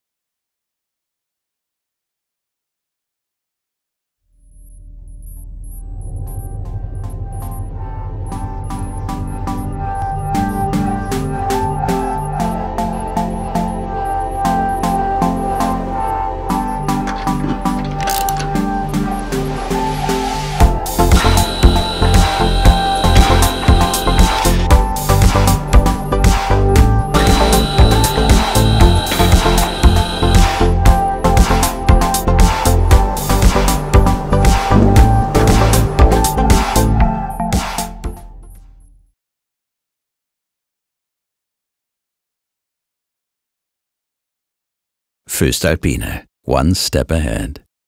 ECOSTAR Point Machine 5.0
3D Animation for the client "voestalpine Railway Systems GmbH"

Music and Intro/Outro by "voestalpine"

The compact electro-hydraulic point machine with external lock.